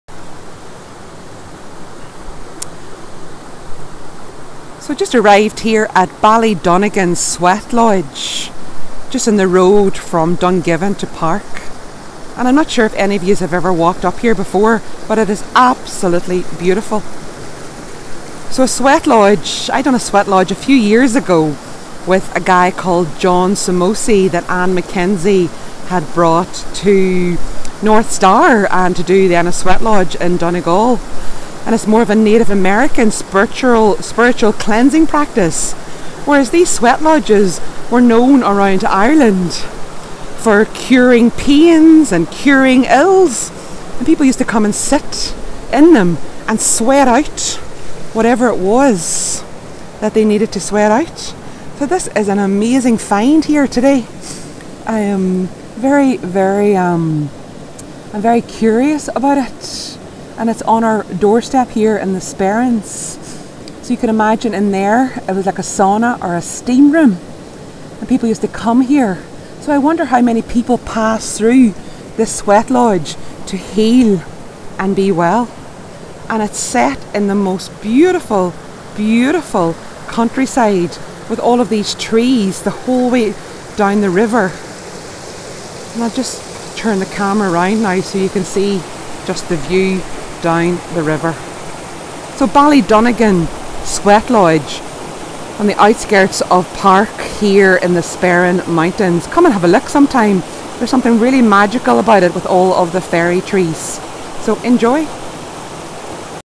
Ballydonegan Sweathouse Sperrins Derry
A beautiful find whilst hiking up Mullaghash in the Sperrin Mountains in Co. Derry.

Part of the North Sperrins Heritage Trail, Ballydonegan Sweathouse is a well preserved example of something quite rare in Ireland. Sweathouses were used like a sauna or steam room to cure fevers and pains. I read on one of the information panels that they can be described as being an early form of Turkish Bath.

Unlike the Native American Sweatlodge that I have experienced, these sweathouses would have had a fireplace of loose stones inside with a stone beam situated above it and a small exit for smoke at the back. Once the fire was lit with turf or wood, the entrance would be sealed to allow the heat to build.

Some rushes were spread on the floor and water thrown on the hot stones to produce steam. A person wishing to use the sweathouse would then enter naked and stay for a while with the door sealed. On exiting, it is said that people would immerse in the waters of the nearby Clougherna Burn flowing from Mullaghash above.

Quite fascinating and definitely worth a visit. Thank you to those who fund this preservation of this monument.